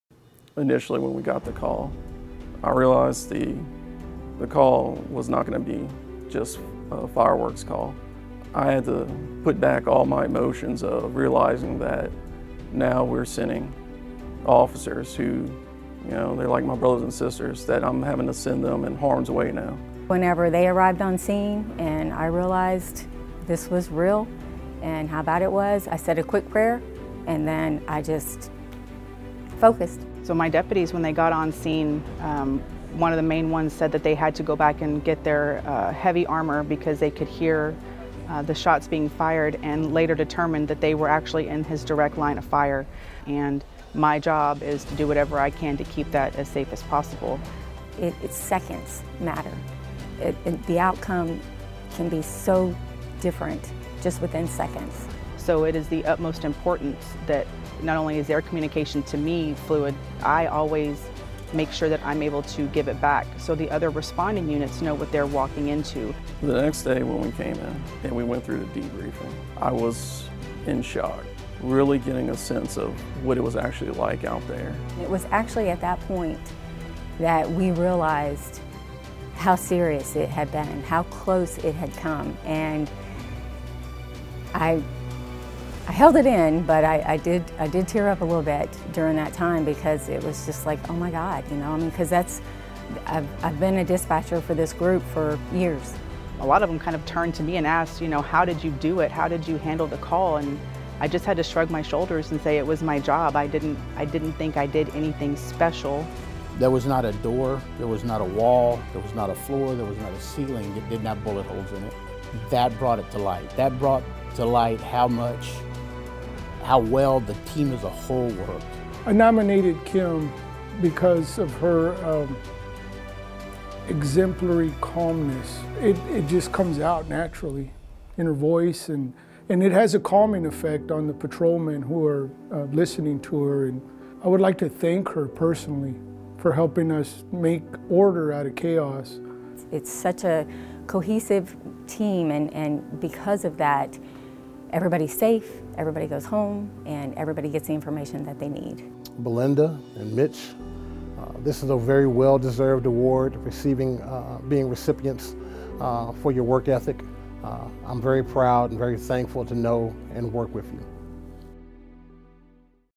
Outstanding Dispatcher 2019 - Pflugerville Police Department and Travis County Sheriff's Office
On August 8, 2018, the Pflugerville Communications Center began receiving 911 calls regarding multiple shots fired at an apartment complex within the city.  The center dispatched officers and Travis County Sheriff's Office responded to assist.  Belinda Alexander, Mitchum Chauvin, and Kimberlea Krenek worked together to coordinate the work of all responding emergency personnel.  They were able to pinpoint the shooter's location and update the units in the field to put an end to the harrowing situation.